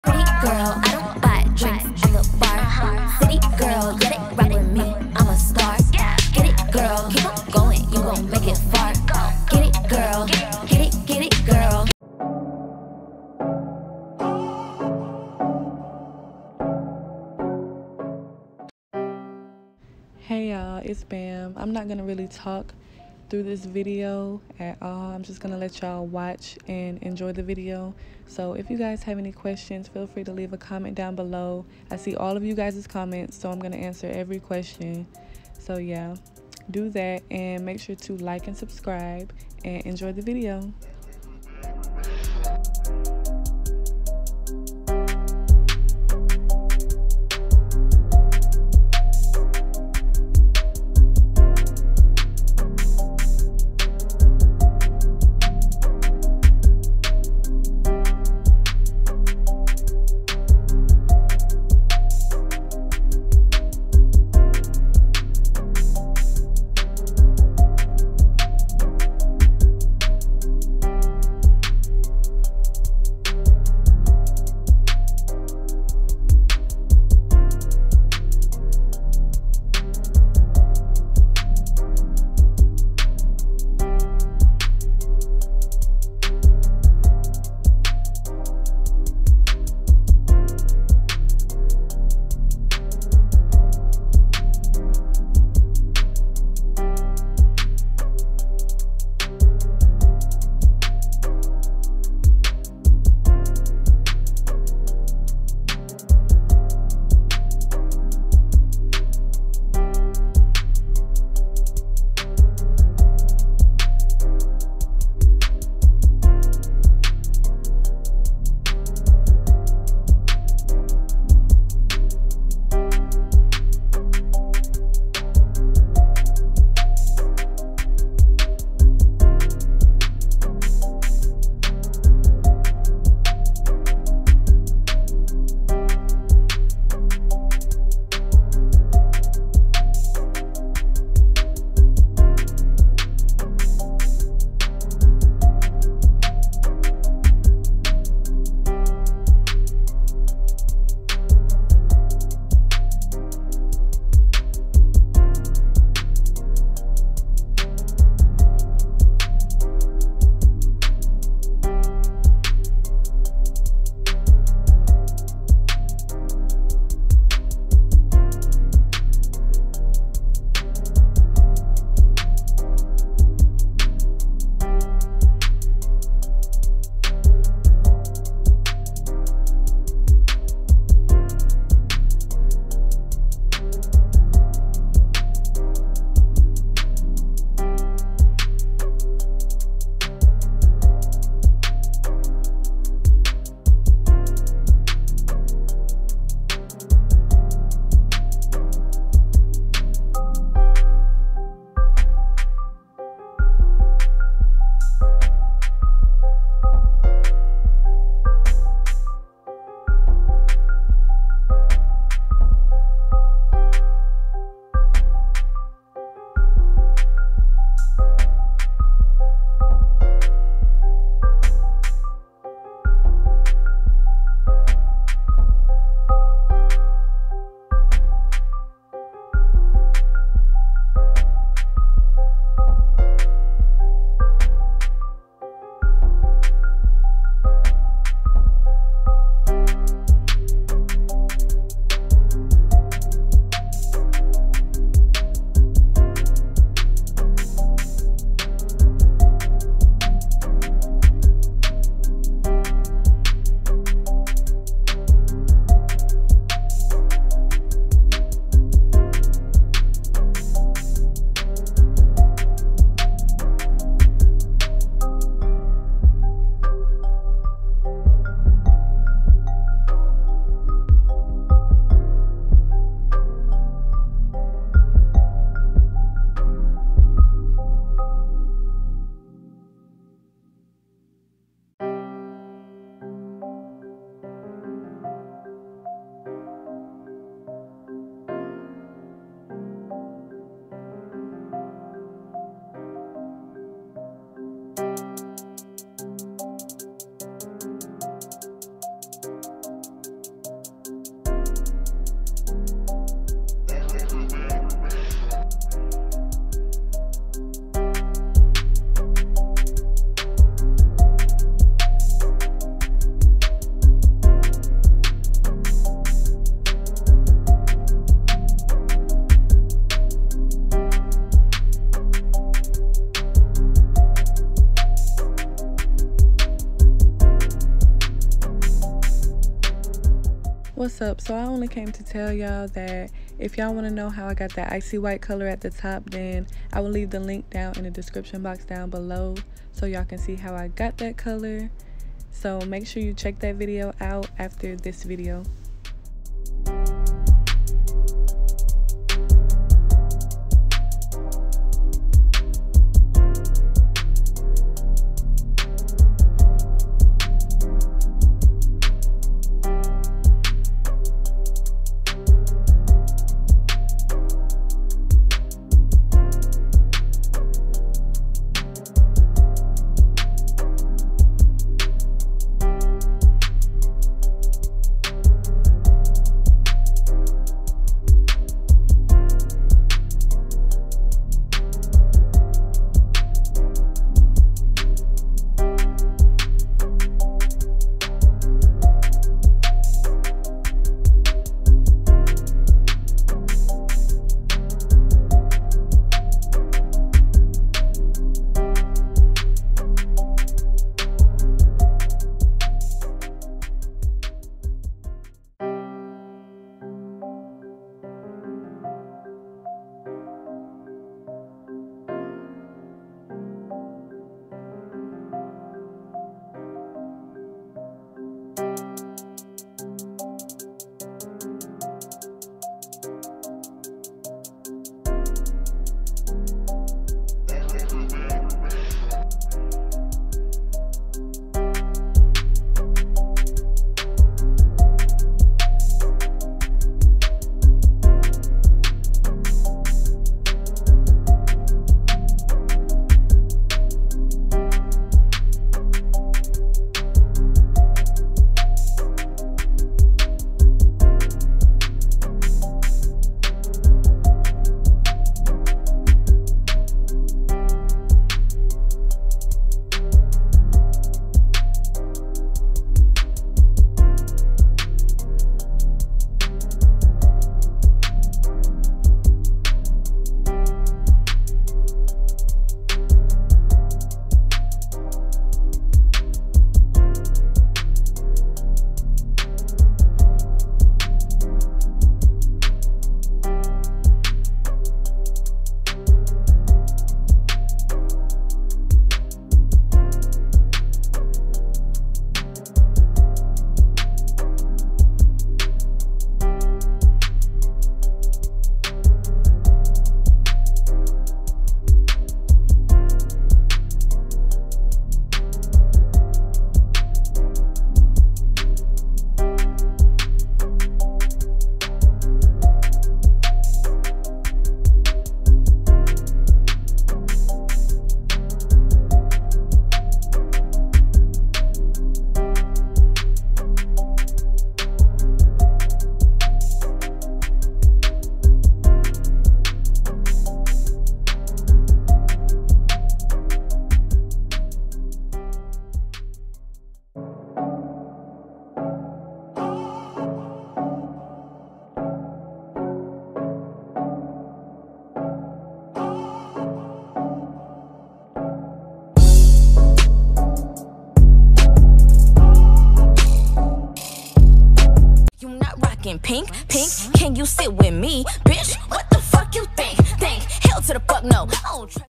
ELSA FROZEN CRAFT 🥶🤍❄️ Ombré Watercolor Method
Wig From | Instagram
@Artsofbamcraft | Dm me to order 💕

Hair Dyes:
- Cyan- Kiss Colors
- Indigo Blue- Adore

Icy White Hair Tutorial🥶⤵️

- Comment “👽🤟🏾🎨” on any of my videos

GOOD LUCK 🖤